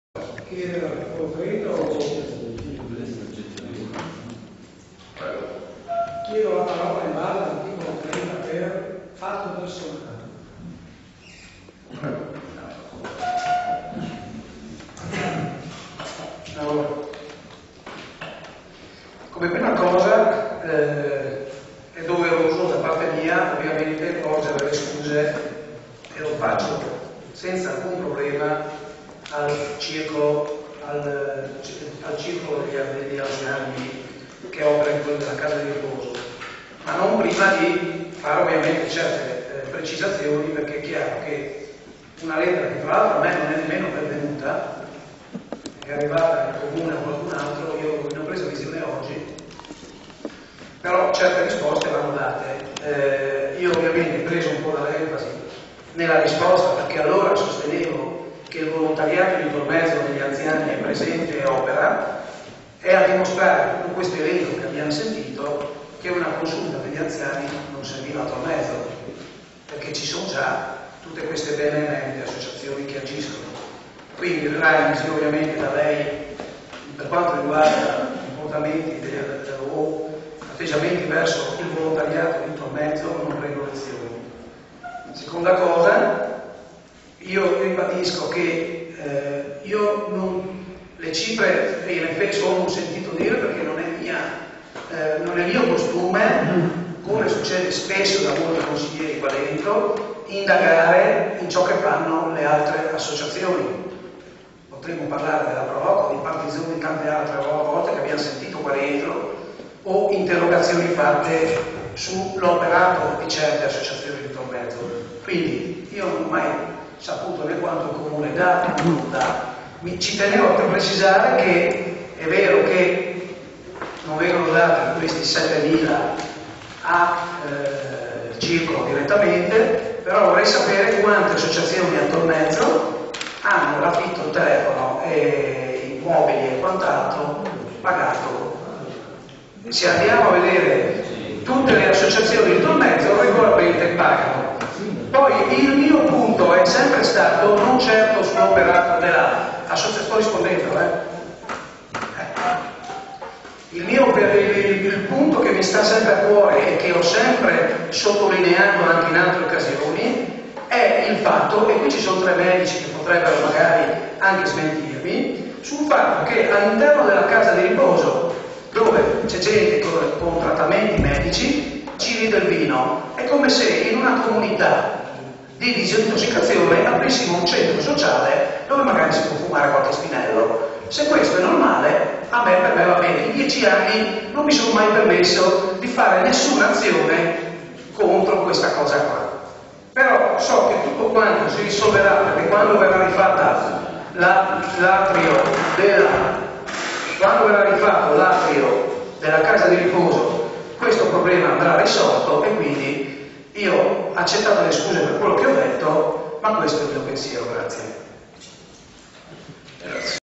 Carnia.La - De Prato chiede scusa al circolo anziani
Renzo de Prato, (Pdl) chiede scusa a Pierino Delicato (presidente del Circolo Centro Diurno per Anziani Città di Tolmezzo) per le affermazioni rese durante il precedente Consiglio comunale, ma ribadisce le proprie tesi di critica alle attività della medesima associazione, quali la somministrazione di vino